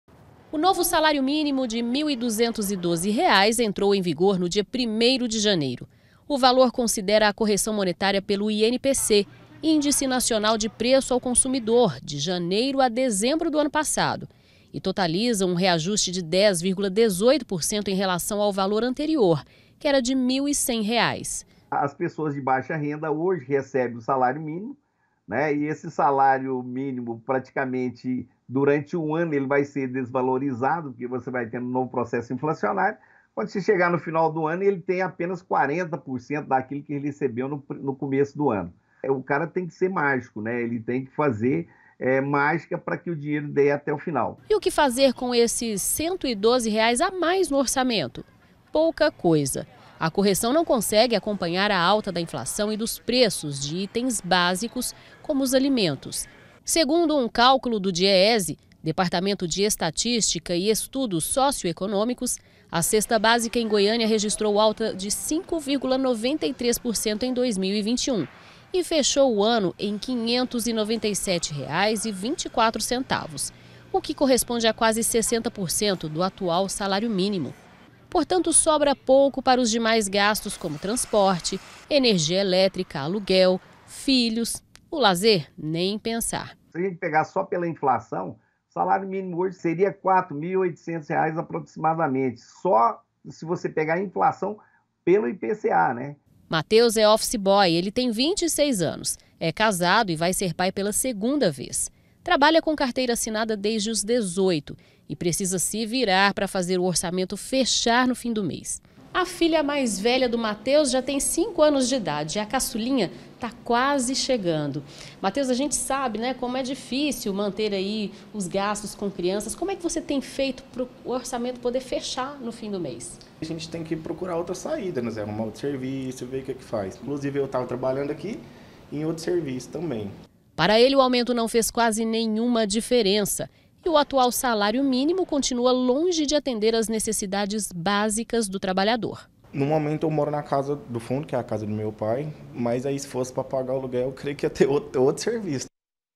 SALÁRIO MÍNIMO: VALOR DEVERIA SER DE R$ 4,8 MIL
O SALÁRIO MINIMO TEM UM NOVO VALOR, MAS AINDA MUITO LONGE DO IDEAL. UM CÁLCULO FEITO POR ECONOMISTAS MOSTRA QUE HOJE NO BRASIL A REMUNERAÇÃO BÁSICA DEVERIA SER DE PELO MENOS QUATRO MIL E OITOCENTOS REAIS, PRA ATENDER AS NECESSIDADES DE UMA FAMILIA COM 4 PESSOAS.